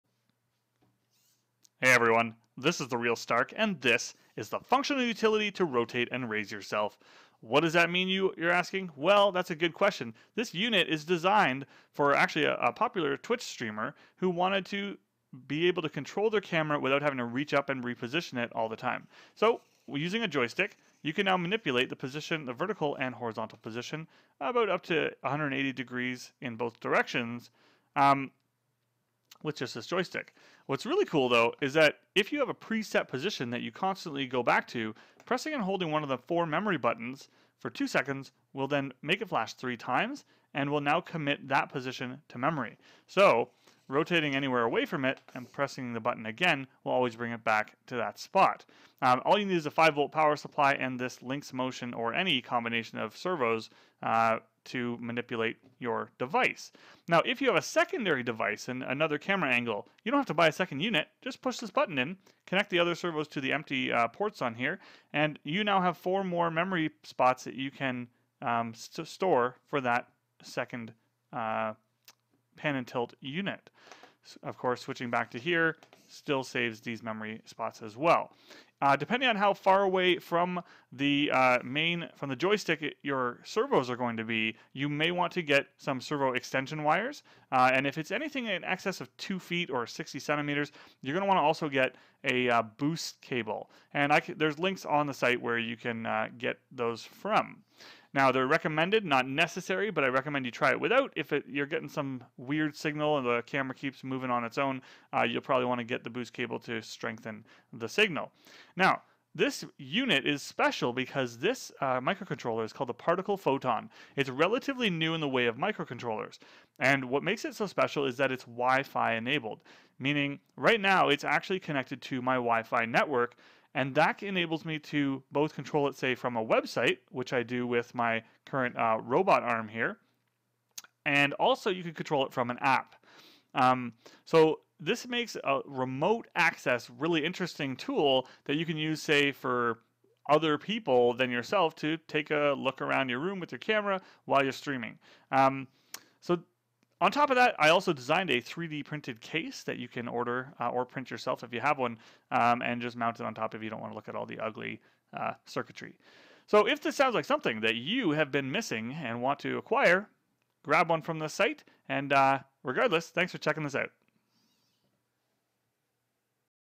Camera Control Unit (Explainer)
An incredible asset to any live-streamer, this Camera Control Unit is equipped to control the pan and tilt of up to two cameras using an embedded joystick.  Clicking the joystick's button will change the on-board indicator LEDs to inform you of which camera you are controlling.

The unit includes four pre-set buttons which, when pressed for 2 seconds, will retain the current position of the camera for quick recall.  Each camera can have up to four independent positions!

How could this get any better?  The WiFi-enabled unit can also be configured to receive commands through the web or even an app.

Oh - and you can even order a 3D printed cover for the unit